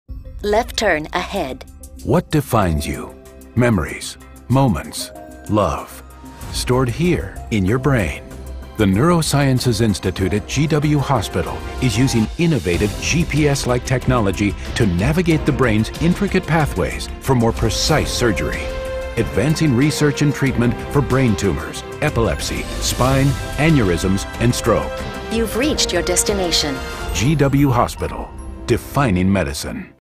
Neurosciences Institute of GW Hospital
Neurosurgeons at GW Hospital are using GPS-like technology to navigate the brain for more precise surgery. The Neurosciences Institute of GW Hospital is advancing research and treatment for brain tumors, epilepsy, spine, aneurysms, AVMs and stroke.

to learn more.